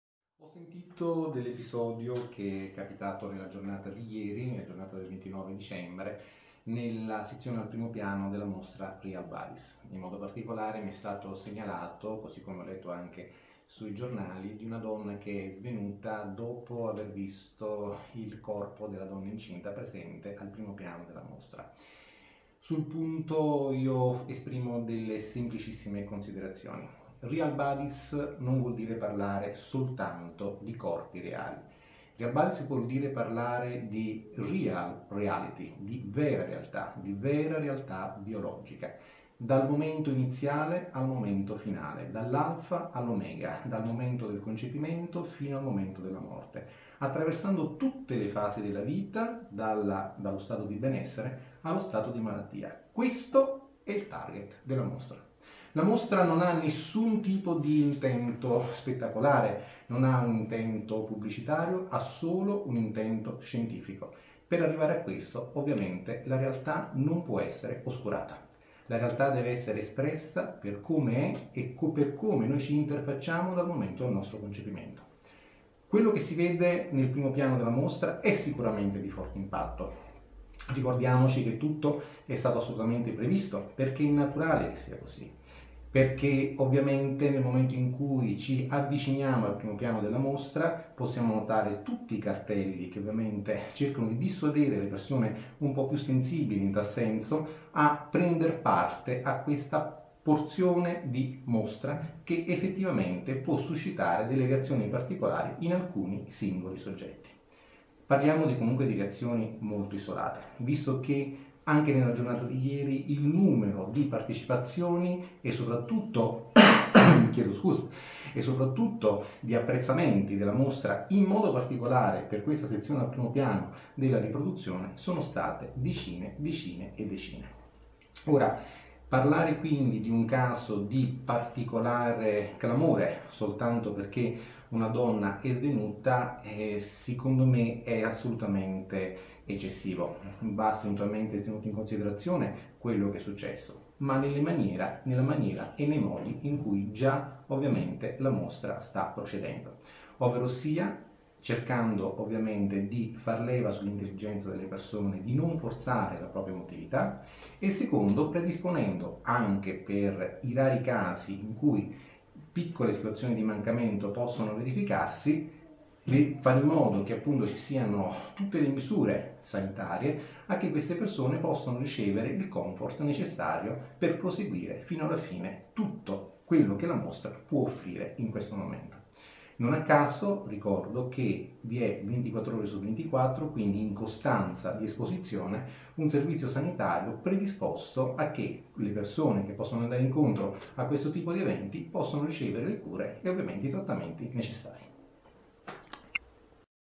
Intervista al Dottor Antonello Cirielli
Trentenne di udine svenuta davanti al plastinato della gestazione, intervista al Dottor Antonello Cirielli